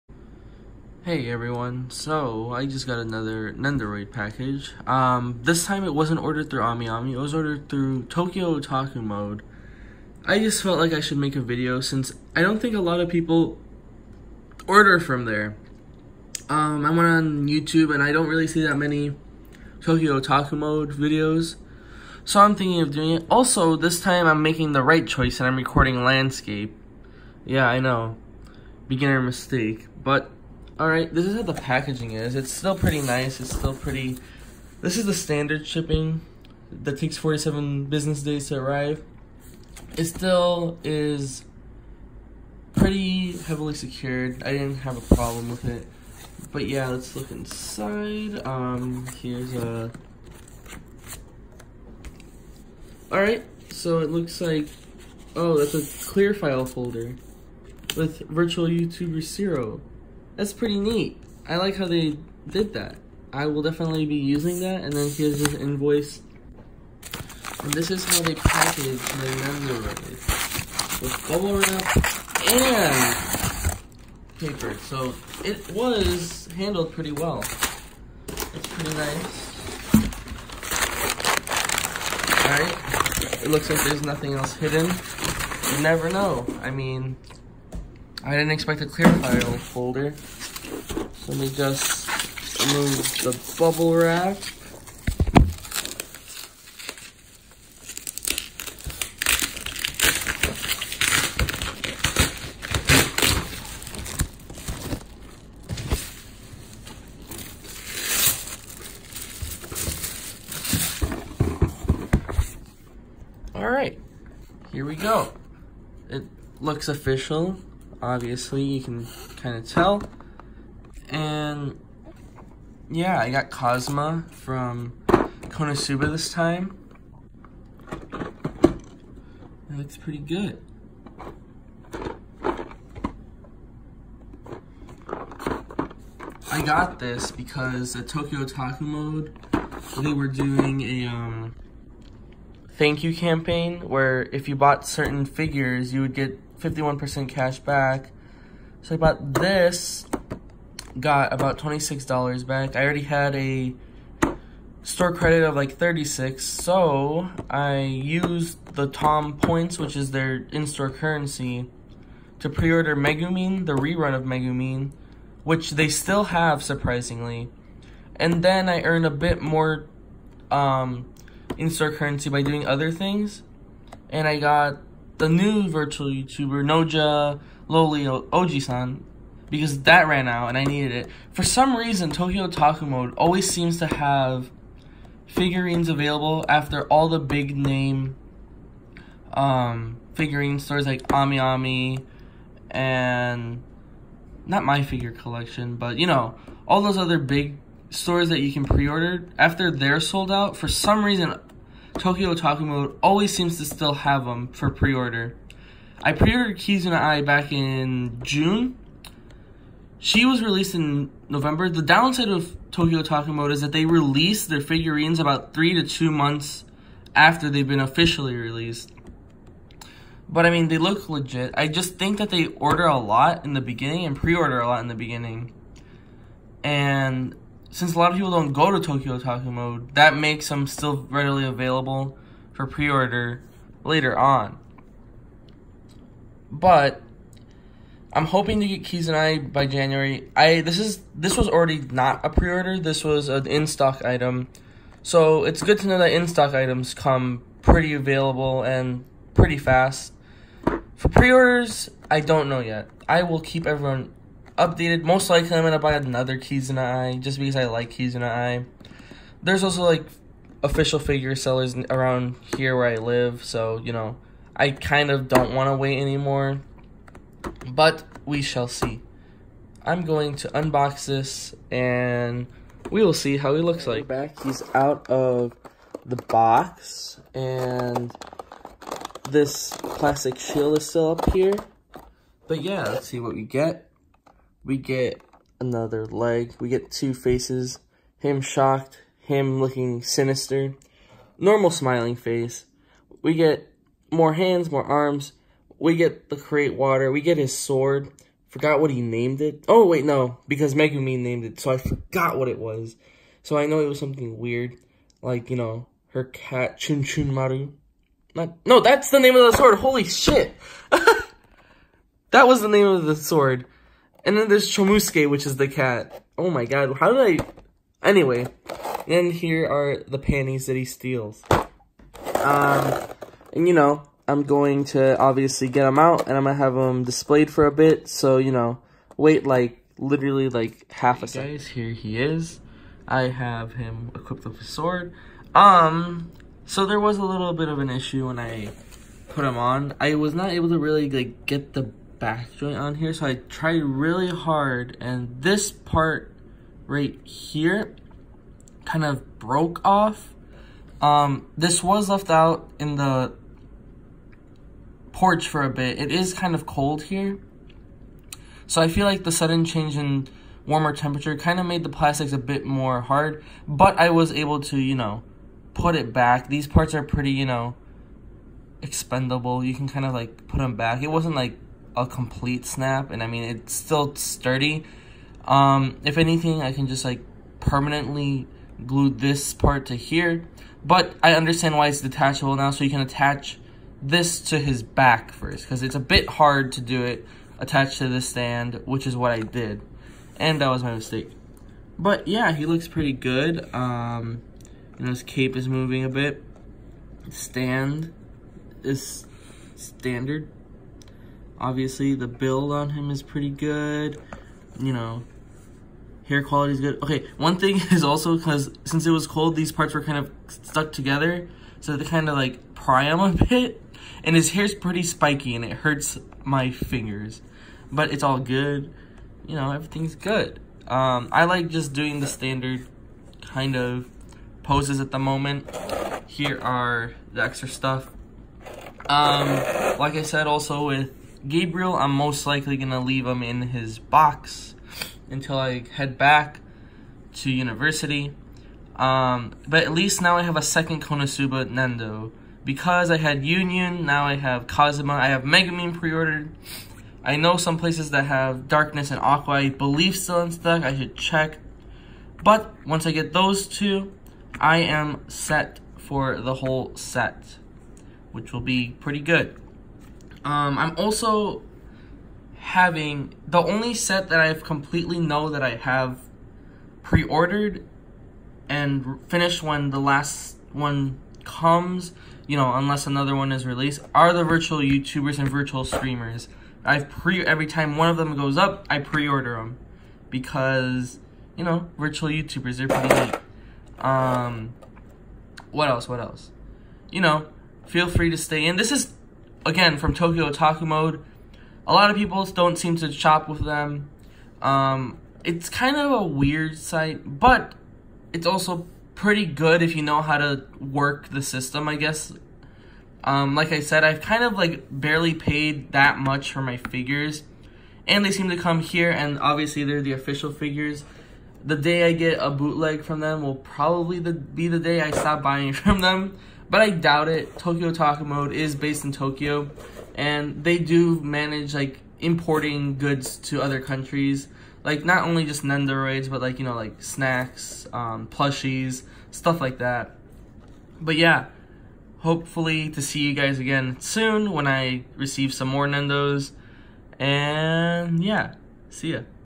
Nendoroid Kazuma
another unboxing of a nendoroid!  this time of kazuma from konosuba!